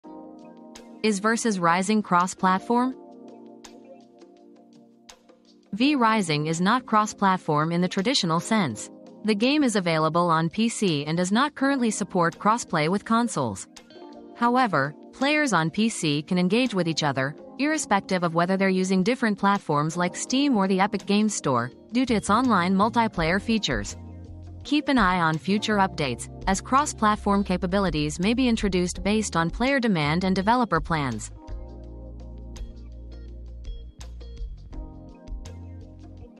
Is V Rising cross-platform?
Is V Rising Cross-Platform? Find Out Now! • V Rising Cross-Play? • Curious if V Rising supports cross-platform play? Discover the current multiplayer options for PC players and what the future might hold for console compatibility!